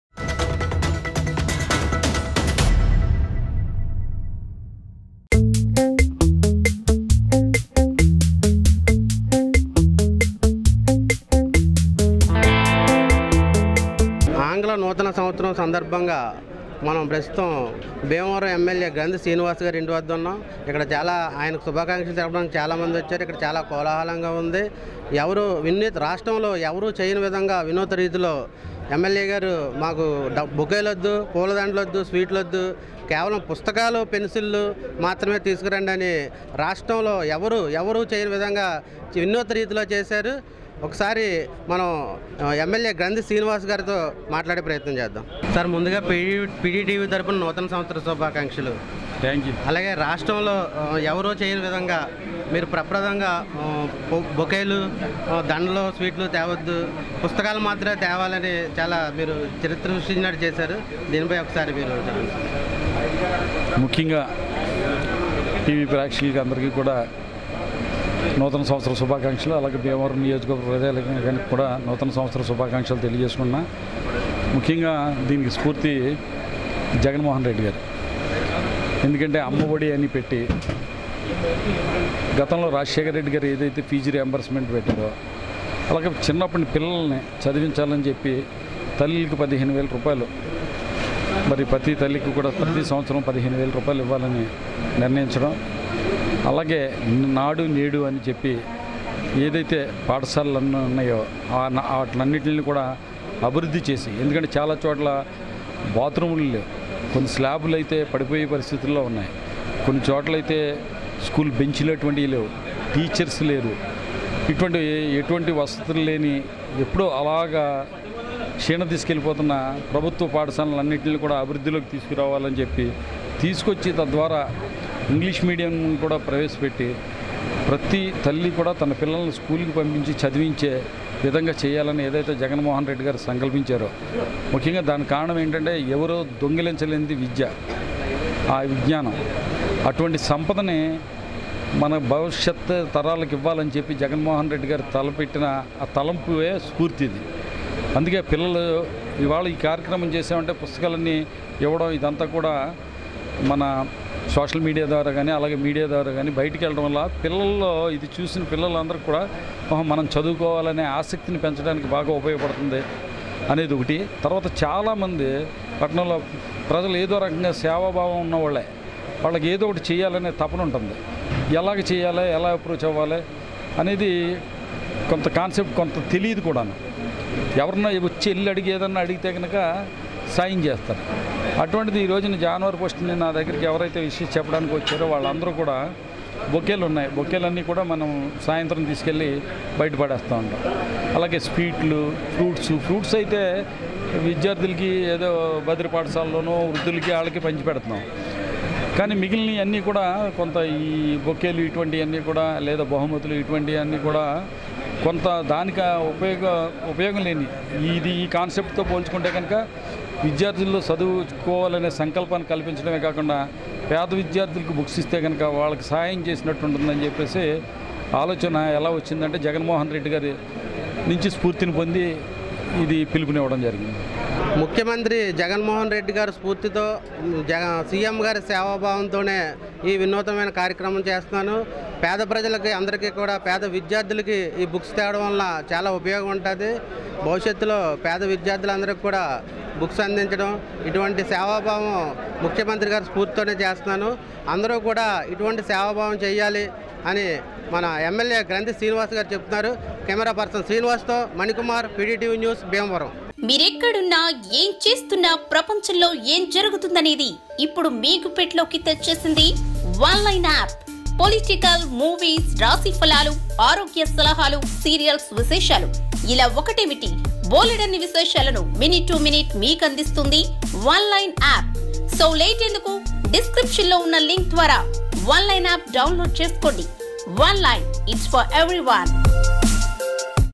MLA Grandhi Srinivas | జగన్ మెచ్చిన ఎమ్మెల్యే బొకేలు,స్వీట్లు వద్దు పుస్తకాలు,పెన్సిల్స్ కావాలి
జగన్ మెచ్చిన ఎమ్మెల్యే బొకేలు, స్వీట్లు వద్దు పుస్తకాలు, పెన్సిల్స్ కావాలి